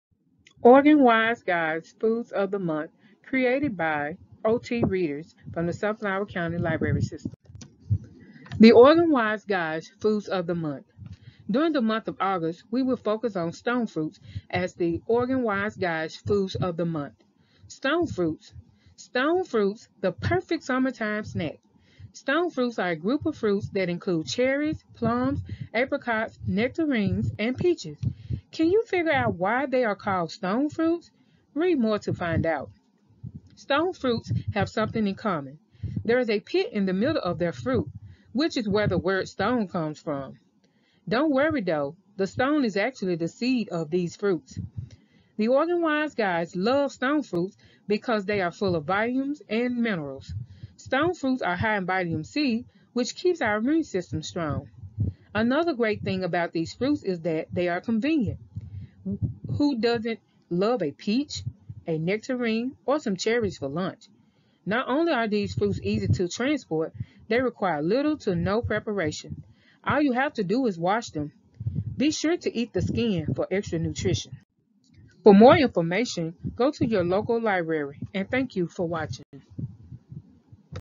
Food of the Month - August 2021 Stone Fruit
Created by the OT Readers of the Sunflower County Library System.  Stop by any branch of the Sunflower County Library System to complete a Patron Feedback Form or to complete it online, go